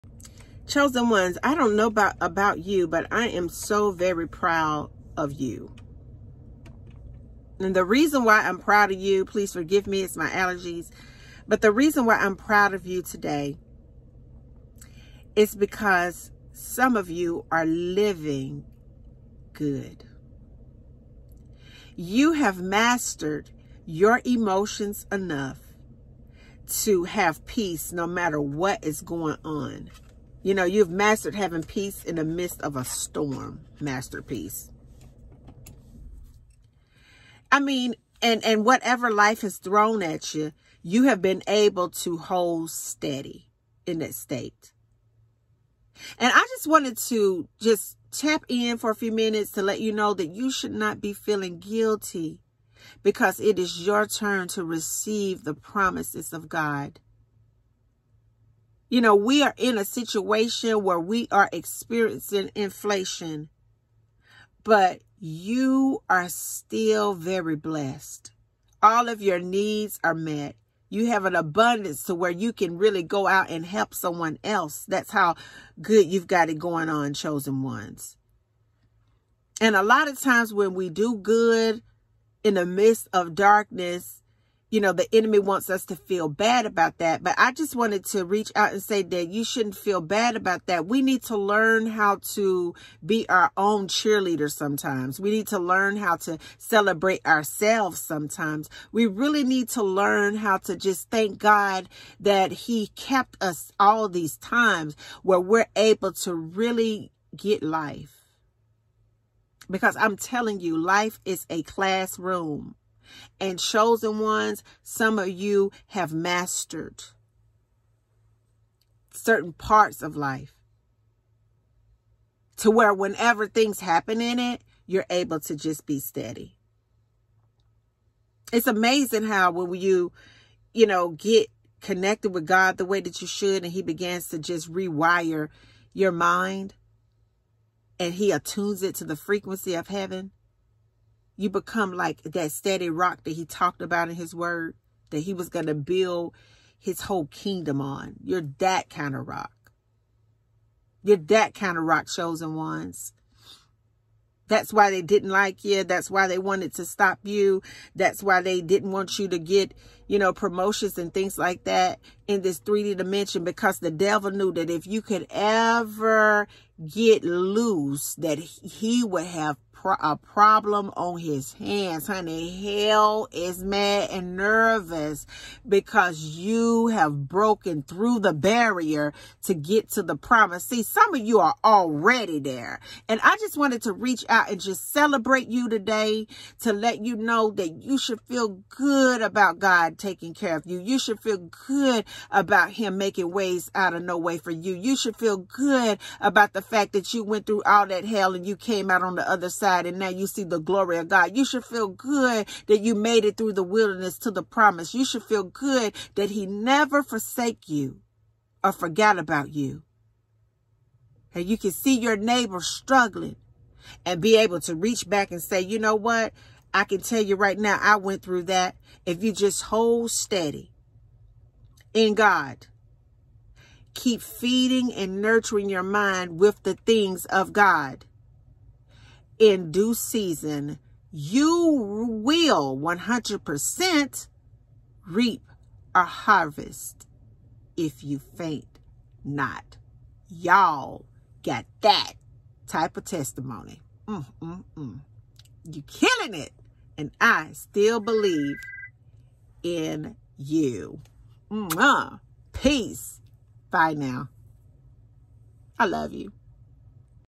🛑 Chosen Ones | 🤡 They Stay Hating and Angry Because Your Blessed | Shonda iNspires
I wanted to drop in to say, “you are amazing and congratulations on all you’ve accomplished!” The enemy is very upset, mad, angry, and hating because of your joy! You are born to win!